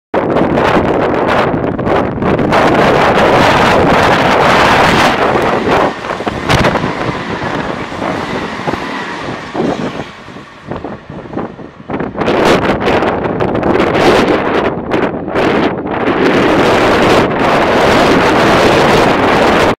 【代走】近鉄5800系DH01編成 急行京都行き 宮津通過
種別『急行』
行き先 京都ゆき
列車番号 第1520列車
撮影日 4月7日
撮影場所 宮津